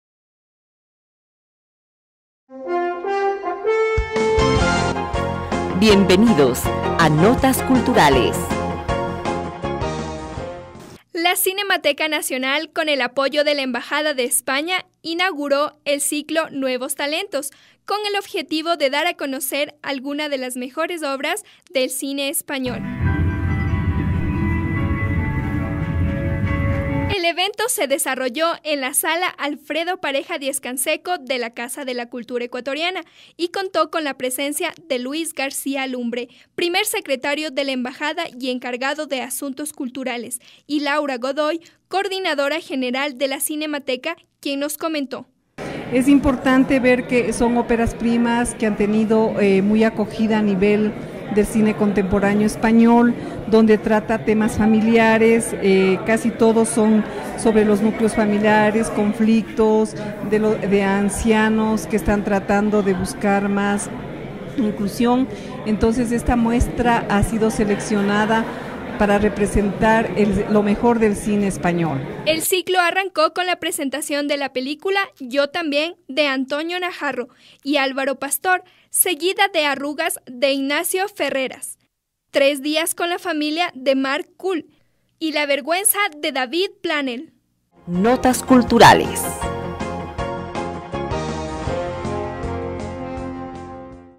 FESTIVAL DE CINE ESPAÑOL
Este año el Ciclo de Cine Español está dedicado a las nuevas promesas del cine español: directores noveles que han producido su primera película en los últimos cinco años y cuyas películas han sido premiadas y reconocidas en el mundo.

Exhibiremos: Arrugas, de Ignacio Ferreras, La vergüenza, de David Planell, Primos, de Daniel Sánchez Arévalo, Tres días con la familia,  de Mar Coll e inauguramos esta muestra con Yo, también, de Álvaro Pastor, Antonio Naharro.